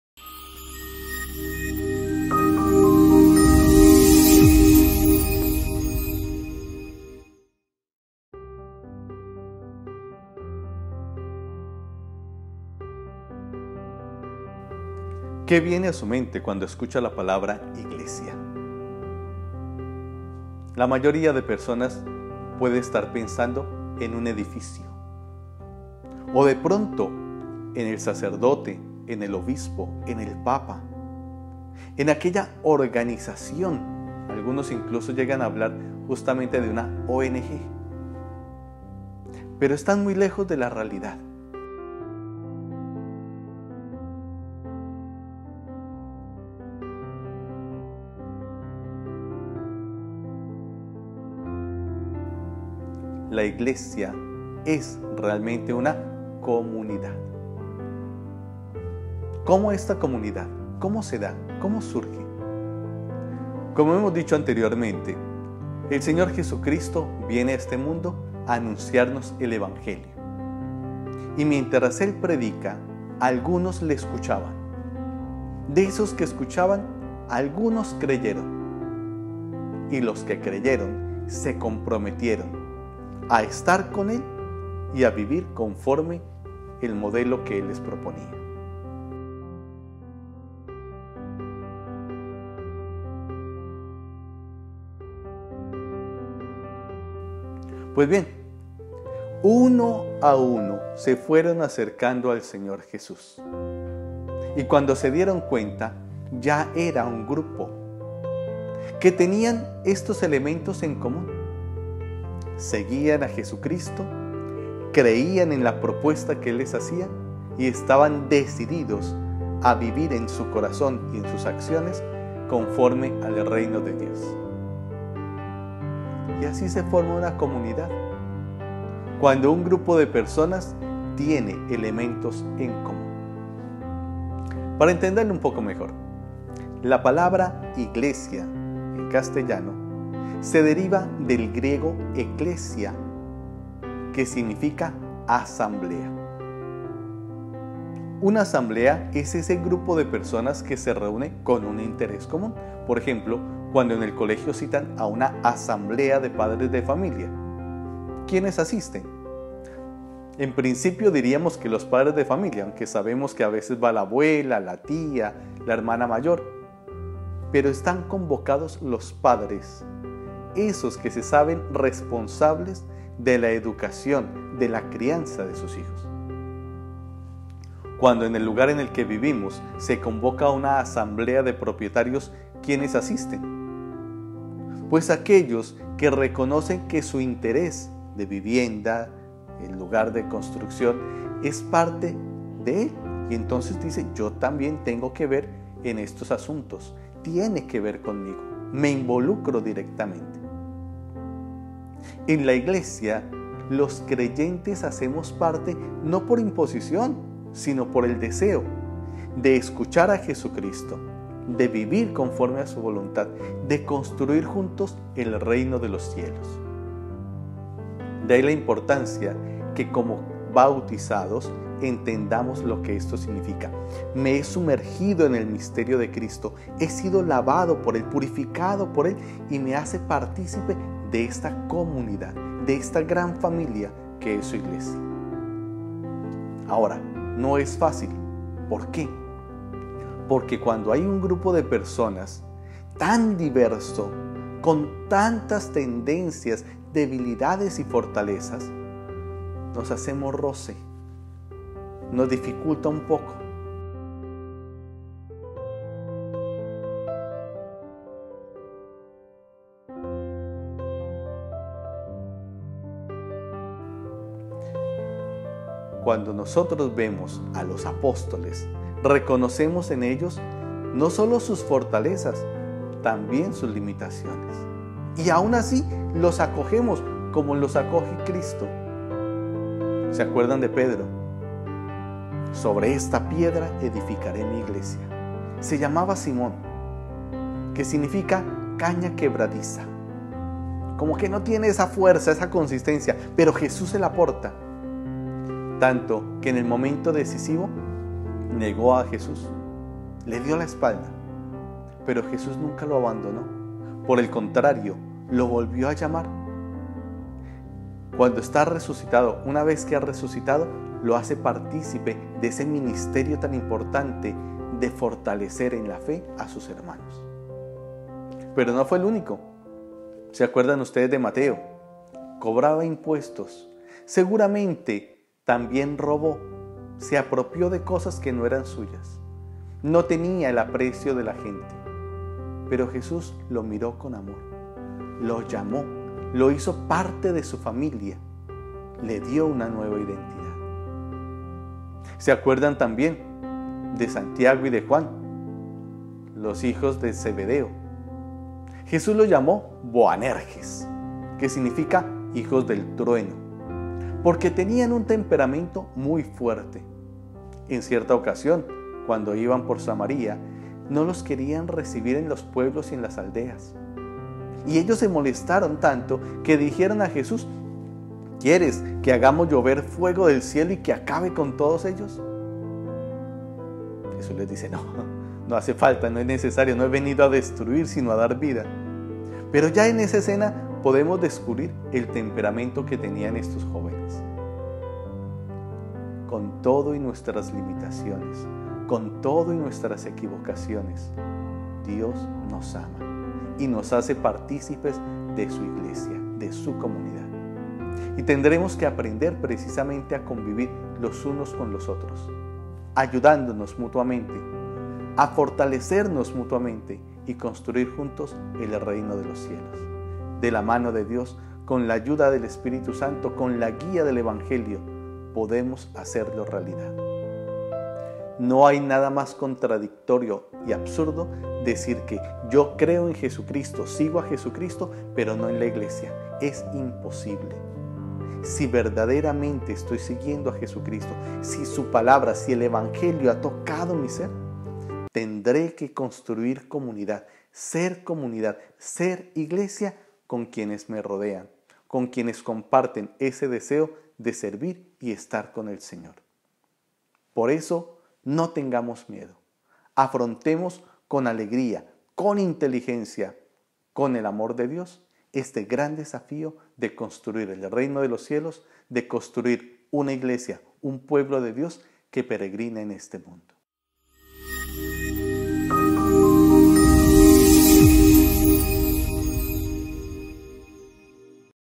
👉 🎯 3 pistas para entender qué es la Iglesia Católica.
Para muchos la Iglesia es una institución  que los sacerdotes dirigen conforme a las enseñanzas del Papa. Otros centran su atención en las "riquezas de la Iglesia" y la identifican con una multinacional que presta ayuda humanitaria. No faltan los que se detienen en las debilidades de los hombres y la identifican como encubridora de pecados.

Todas son lecturas que se corresponden con experiencias buenas o malas que la gente ha tenido de cara a la Iglesia, pero pocos se atreven a profundizar en su significado. ¿Qué es la Iglesia Católica? ¿Cuál es su razón de ser? ¿Quiénes la conforman?

Ofrecemos algunas líneas de interpretación que abren el panorama de la eclesiología y su significado.